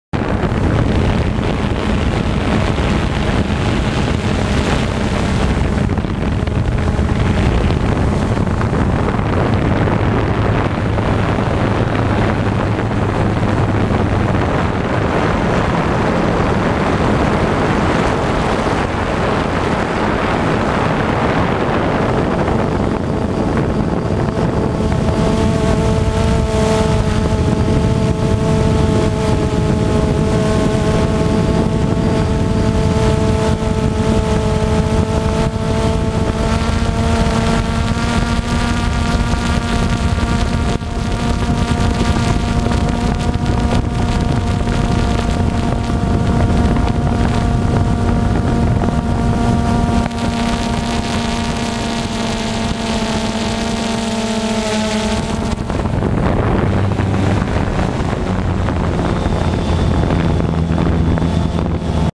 Circling the Greenpoint Water Tower
Practicing  Circling the Greenpoint Water Tower with my DJI Phantom